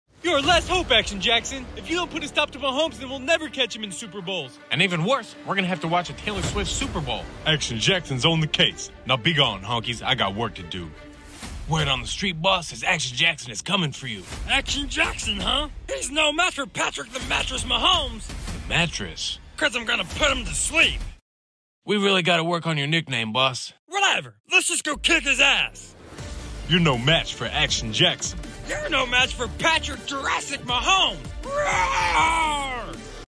Lamar “Action” Jackson and Patrick Mahomes Face-Off 😂 Chiefs Vs Ravens
Lamar “Action” Jackson and Patrick Mahomes Face off 😤😂 NFL Cartoon Parody - Ravens Vs Chiefs